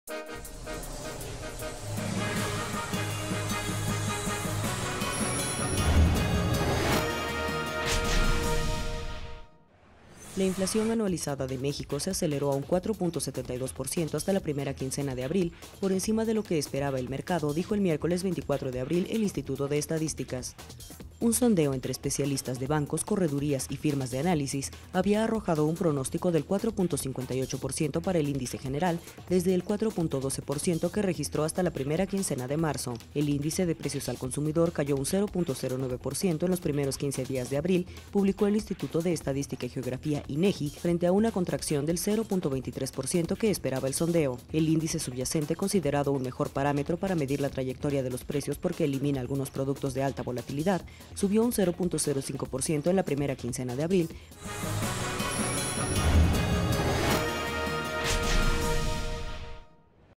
Corte 44: Inflación anualizada de México se aceleró en un 4.72%
La inflación anualizada de México se aceleró en un 4.72%, hasta la primera quincena de abril.